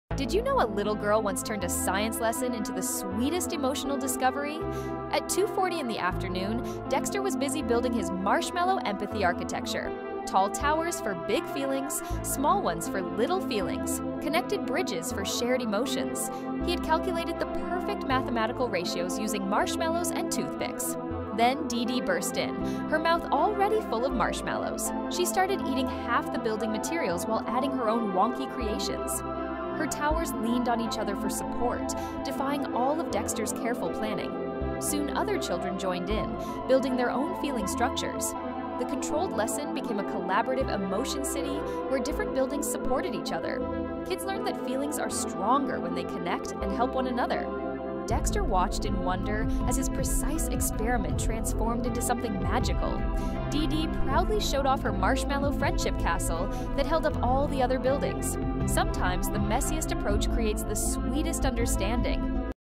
When Kids Turn Science Into Pure Magic: Marshmallow Empathy Towers
marshmallow tower chaos becomes friendship lesson 🥺